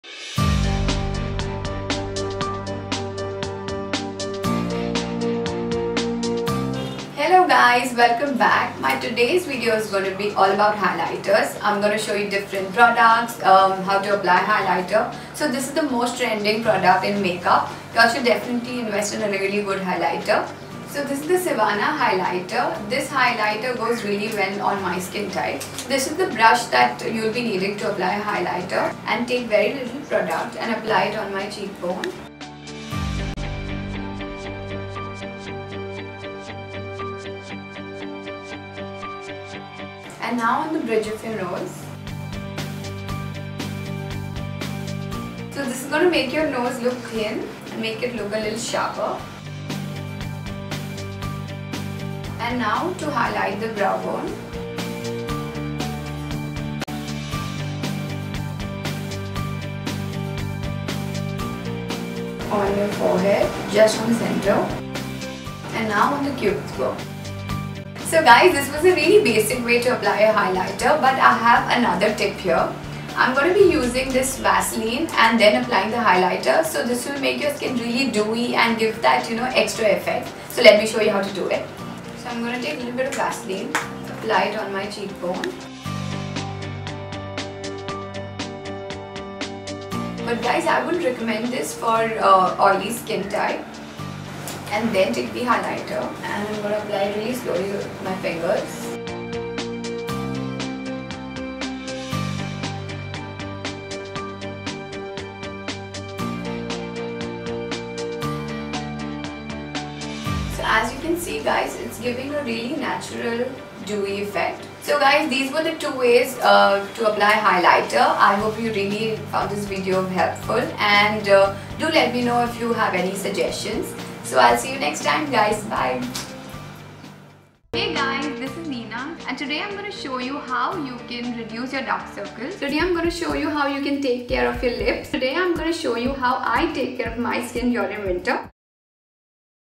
Learn How To Use Highlighter | Make Up Kit | Beginners | Make Up Videos | Foxy
Welcome to foxy

In this video we will be showing you how to use highlighter